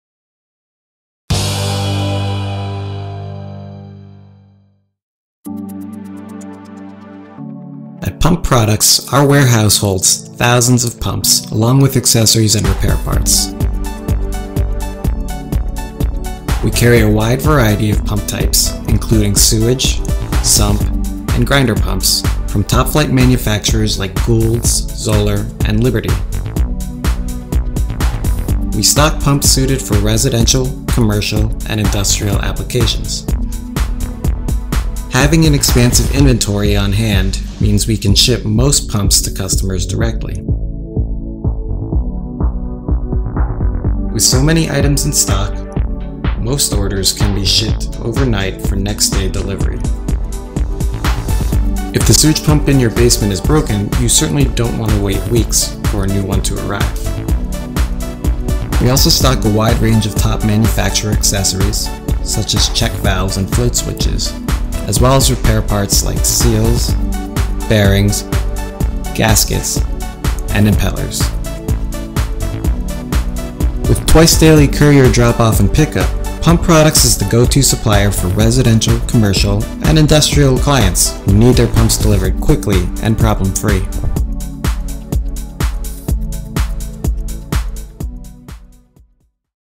Pump Products Has the Inventory to Deliver Your Pump Faster
Some ecommerce companies don't put too much stock in inventory. Pump Products has a warehouse with thousands of items in stock. What does that mean for customers? We can get your pump or part to your faster. If you need a new pump don't wait and don't hesitate.